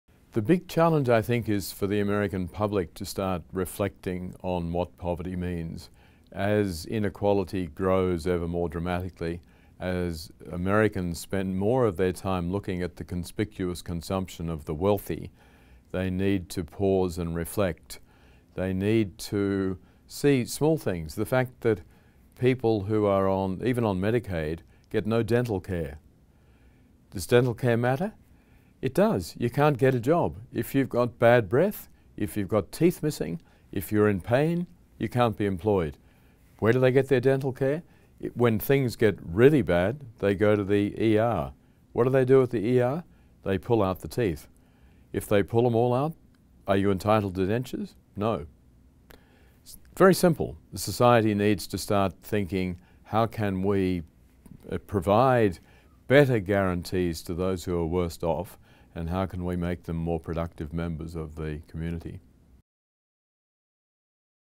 How can Americans address widespread poverty and increasing income inequality?
Learn more about Professor Philip Alston and his work at NYU Law